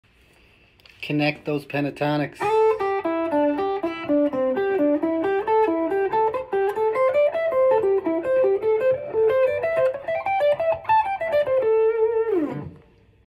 Connecting the Pentatonic shapes - economy picking - Free Tab
Check out this handout booklet I made for my students.
Major & Minor Pentatonic scale - in 12 keys around the Circle of Fifths.

Knowing the lowest possible pentatonic shape for every key is essential. Break out of the box that everyone tends to use. This allows you to switch scales without changing positions and provides a deeper understanding of the neck. Instead of jumping to the box your formular with, you can follow the chord changes confidently without changing positions.